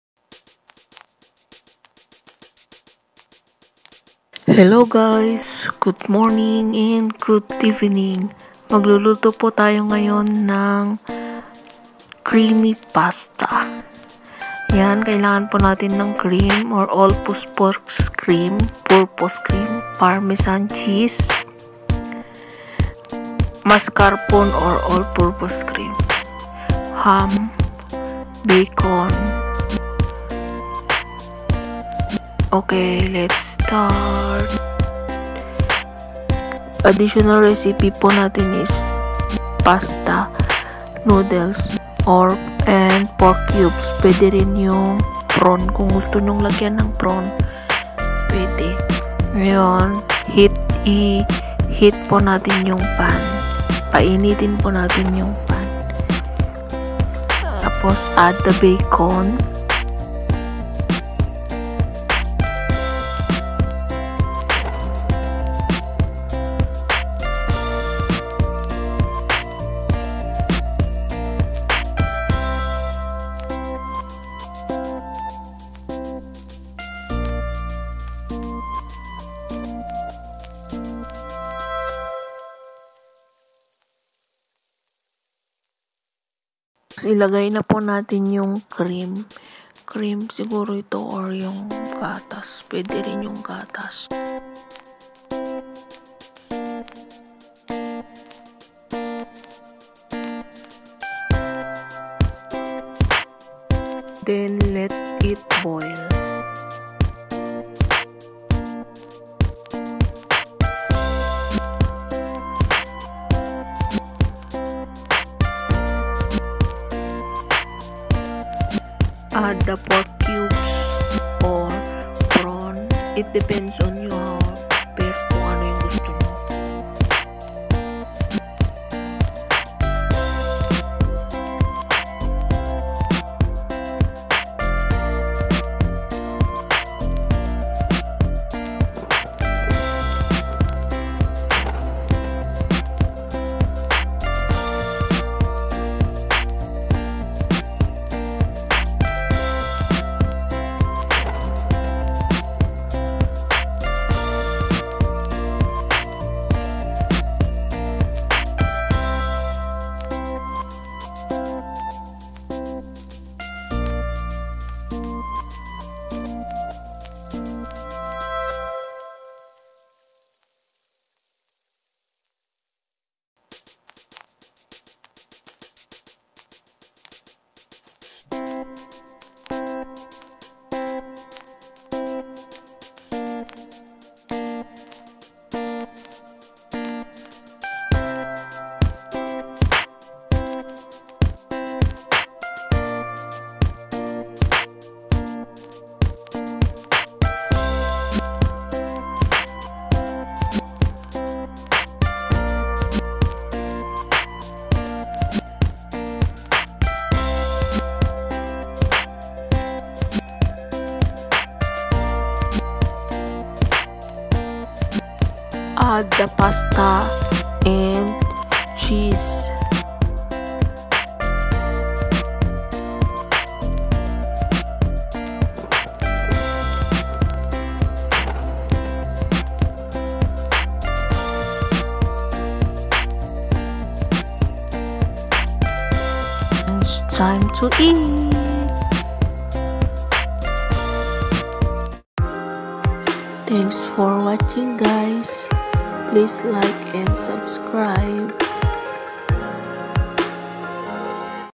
EUROPEAN CARBONARA
FOR BETTER VIDEO PLEASE TURN YOUR SETTINGS INTO HD.

CARBONARA RECIPE
*all purpose cream,milk, cheese,pasta, bacon/ham, pork

kinemaster music

VIDEO EDIT: KINEMASTER/INTROMAKER/ VIVAVIDEO
CAMERA : CELLPHONE